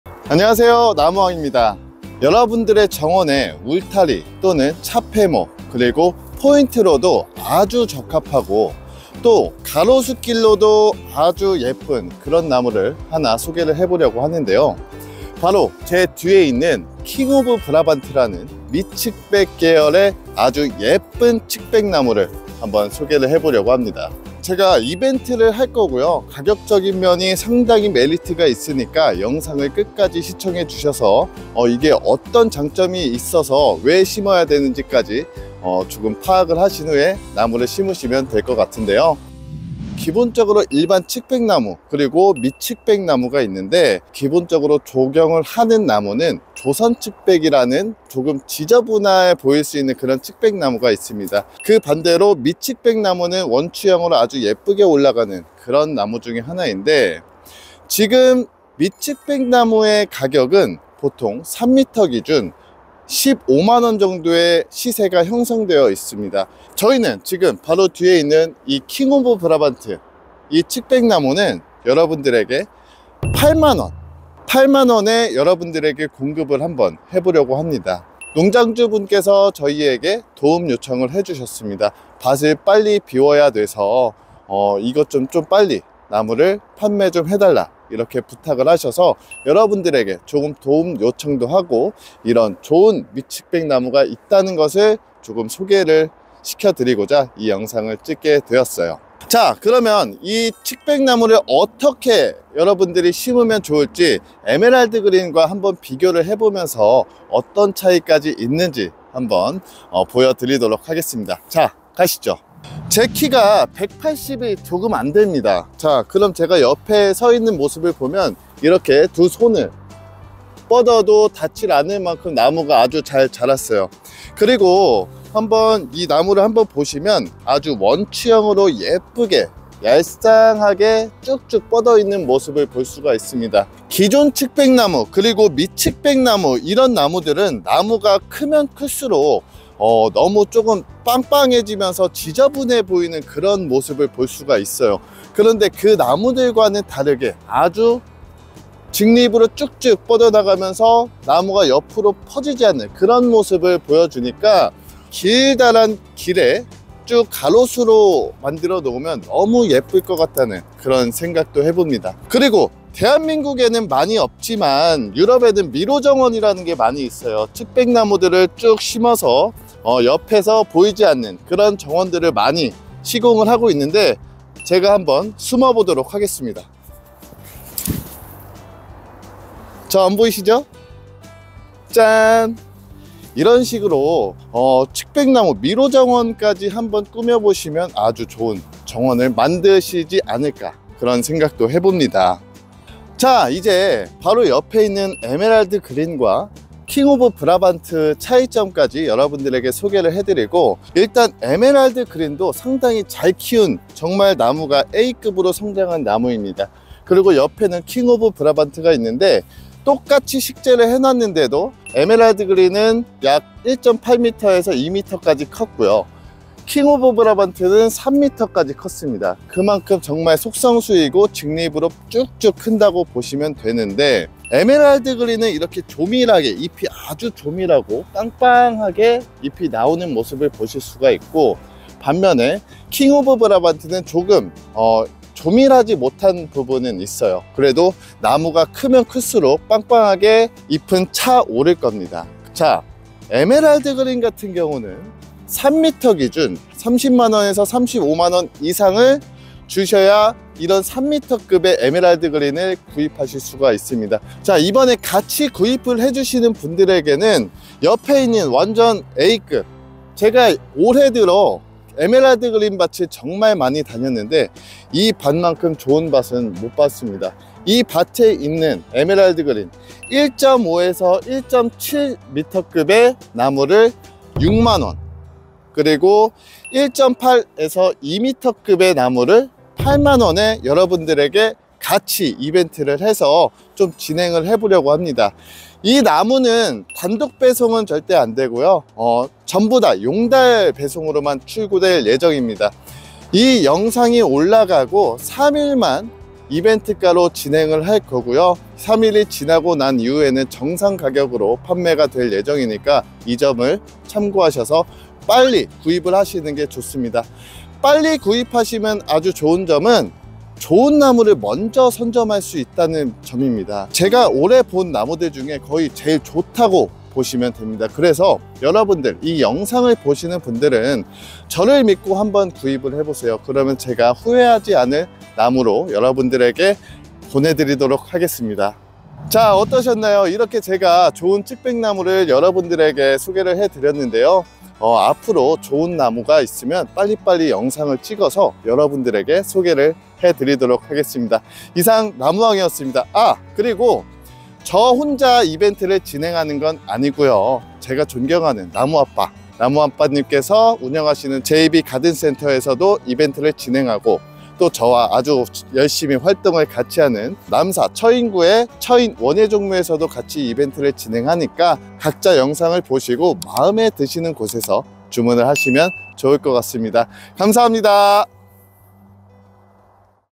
( 단3일! 특별 이벤트 )생울타리 및 가로수로 안성맞춤! 킹오브 브라반트 측백나무 소개
안녕하세요 :) 구독자 여러분 '나무왕' 입니다.
이번 영상에서는 울타리 및 포인트목으로 안성 맞춤인 킹오브 브라반트에 대해서 소개해 보도록 하겠습니다. 브라반트 계열은 성장속도가 빨라 빠르게 울타리 형성이 가능한데요.
그래서 생울타리를 생각하시는 분들에게는 아주 좋은 품종이지 않을까 싶습니다.
영상에서 자세한 설명을 확인해 보시고 특별 이벤트 놓치지 않으셨으면 좋겠습니다!

⭐단3일! 특별 이벤트 킹오브 브라반트 구매 하러 가기⭐

❤놓칠 수 없는! 에버그린농원 이벤트 품목 지금 당장 보러가기!👇👇👇
🔻대박 이벤트 품목🔻